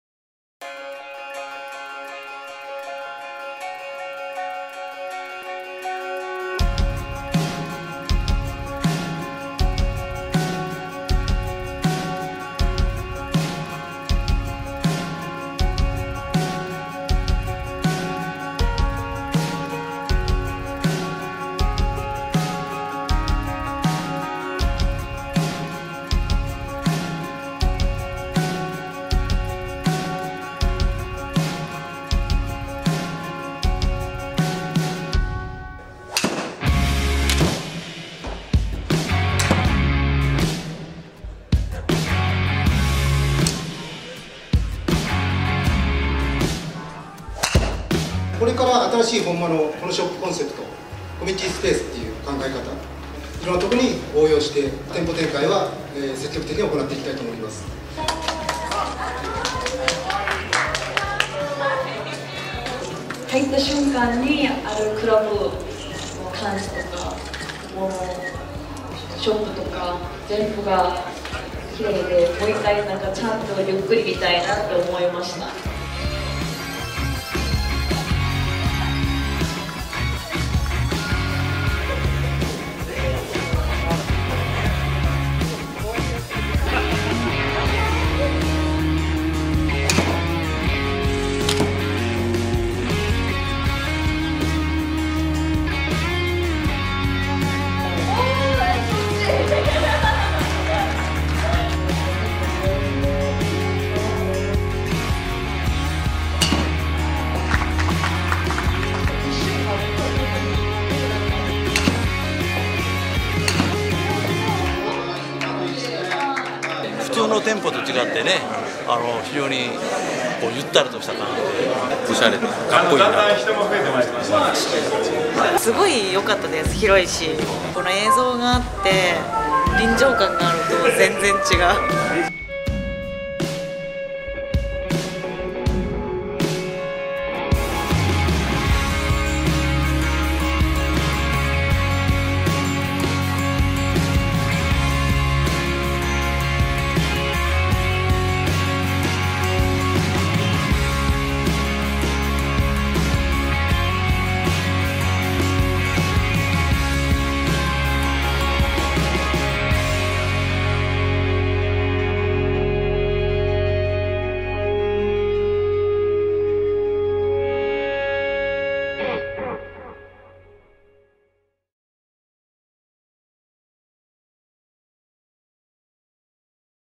本間ゴルフ新橋銀座口店オープニングイベント
本間ゴルフの新旗艦店「新橋銀座口店」が2019/6オープン！
オープニングイベントにはイボミプロがゲストとして参加。レッスン会やシミュレーションゴルフの実演等おこなっていただきました。
新橋駅から信号を1つ渡るだけのアクセス抜群の立地に加え、日本最大級のモニタサイズを誇るシミュレーションゴルフや、酒田工場の「匠」が常駐、などなどゴルフの上達をフォローいたします。
是非皆さまご来店ください。